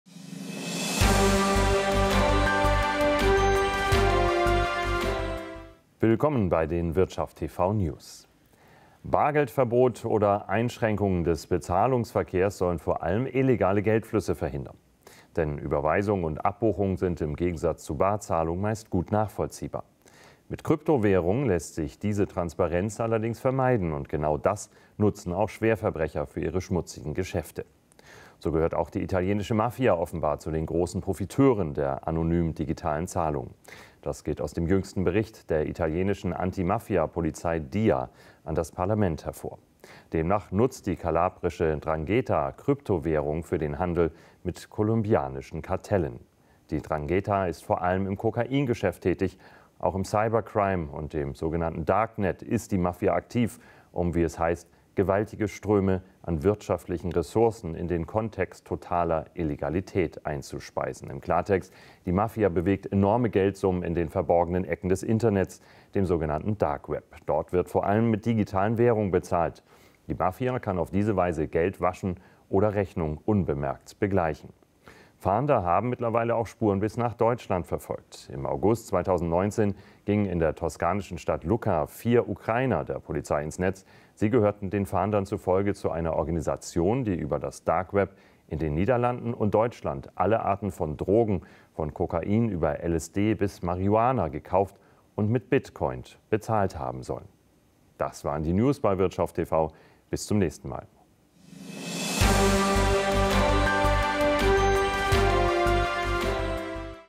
Mafiosi nutzen Kryptowährungen für schmutzige Geschäfte
Bargeldverbot oder Einschränkungen des Barzahlungsverkehrs sollen vor allem illegale Geldflüsse verhindern. Denn Überweisungen und Abbuchungen sind im Gegensatz zu Barzahlungen meist gut nachvollziehbar. Mit Kryptowährungen lässt sich diese Transparenz allerdings vermeiden. Und genau das nutzen auch Schwerverbrecher für ihre schmutzigen Geschäfte. So gehört auch die italienische Mafia offenbar zu den großen Profiteuren der anonymen digitalen Zahlungen.
Das geht aus dem jüngsten Bericht der italienischen Anti-Mafia-Polizei Dia an das Parlament hervor. Demnach nutzt die kalabrische ’Ndrangheta Kryptowährungen für den Handel mit kolumbianischen Kartellen. Die ’Ndrangheta ist vor allem im Kokain-Geschäft tätig.
Auch im Cybercrime und dem sogenannten Darknet ist die Mafia aktiv, um, wie es heißt, gewaltige Ströme an wirtschaftlichen Ressourcen in den Kontext totaler Illegalität einzuspeisen.
Im Klartext: Die Mafia bewegt enorme Geldsummen in den verborgenen Ecken des Internets, dem sogenannten Dark Web. Dort wird vor allem mit digitalen Währungen bezahlt. Die Mafia kann auf diese Weise Geld waschen oder Rechnungen unbemerkt begleichen.
Fahnder haben mittlerweile auch Spuren bis nach Deutschland verfolgt. Im August 2019 gingen in der toskanischen Stadt Lucca vier Ukrainer der Polizei ins Netz. Sie gehörten den Fahndern zufolge zu einer Organisation, die über das Dark Web in den Niederlanden und Deutschland alle Arten von Drogen von Kokain über LSD bis Marihuana gekauft und mit Bitcoin bezahlt haben soll.
Moderation: Wirtschaft TV Moderator Sascha Oliver Martin
Bildquellen: